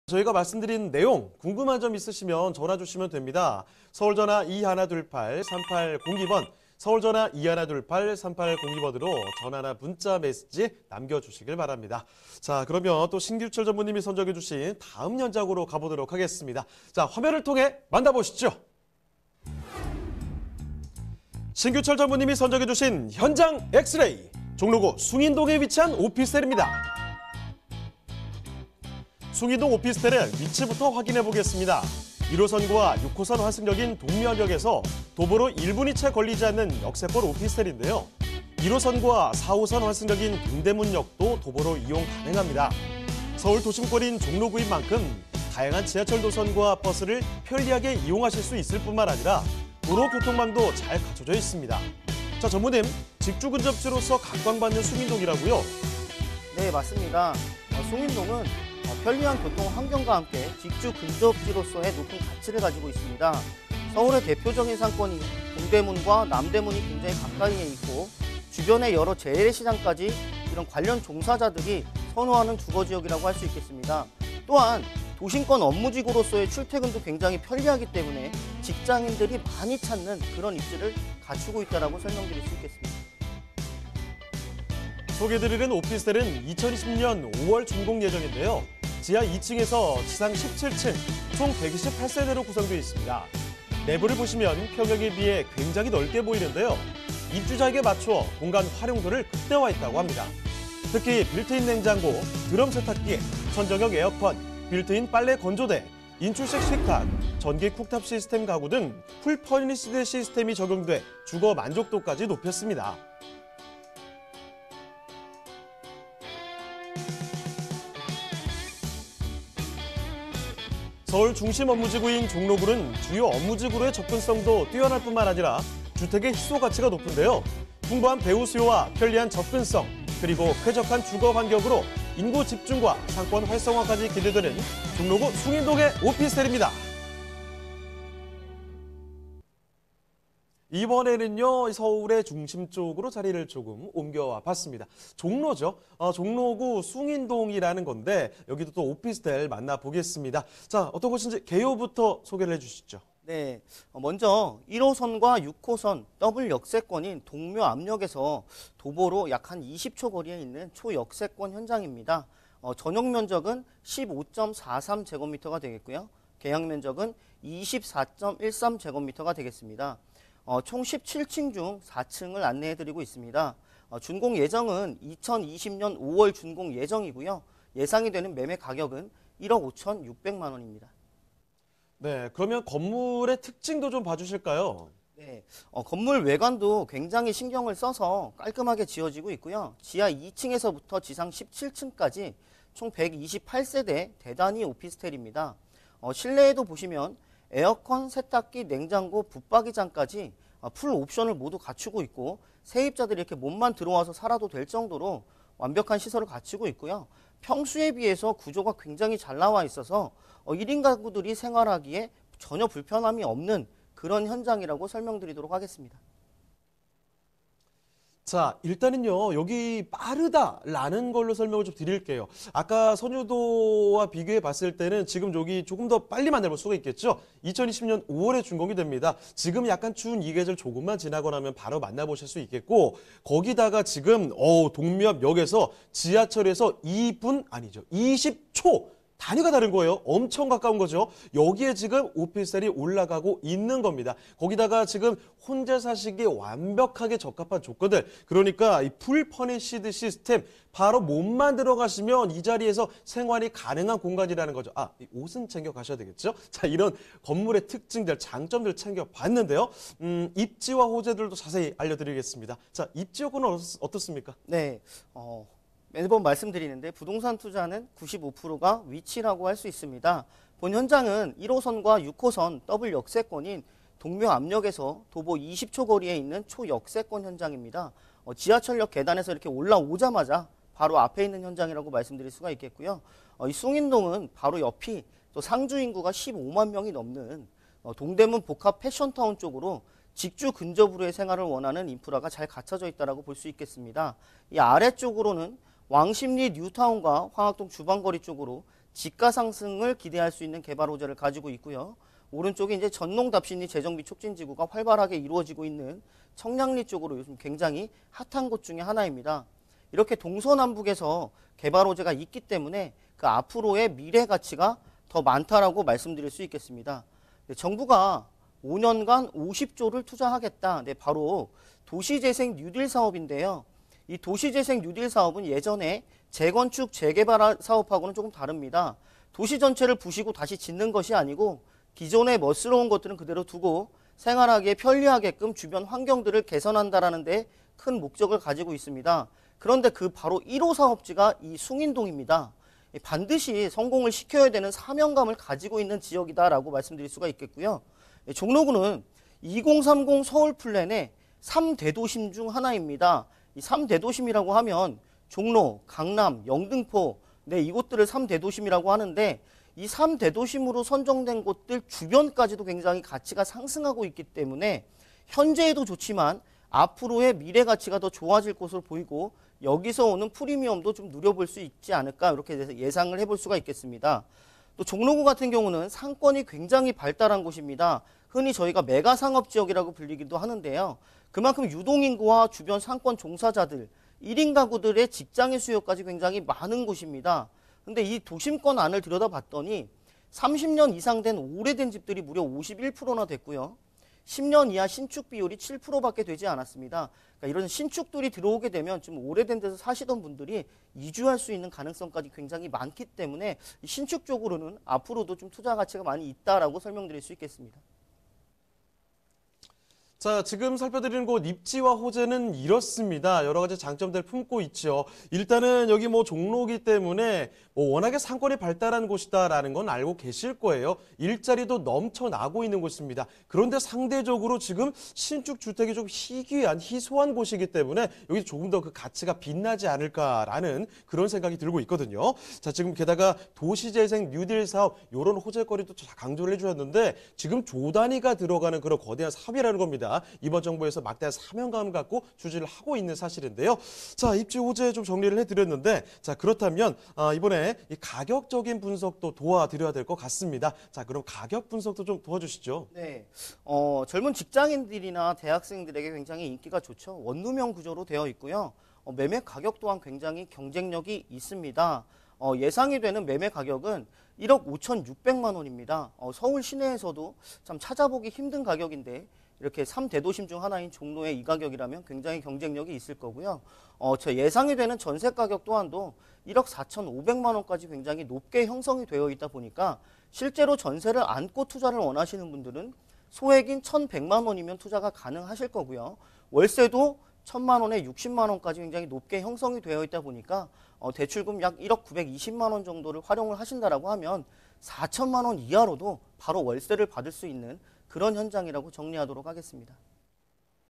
부동산토마토TV 알토마토 rtomato 12/04(수) 현장 X-RAY : 1ㆍ6호선 동묘앞역 도보 약 20초 종로구 숭인동 오피스텔
신규철 전무(U&S하우징)가 소개하는
종로구 숭인동 오피스텔

생방송 – 현장X-RAY - 월~금 오전 11:00~11:30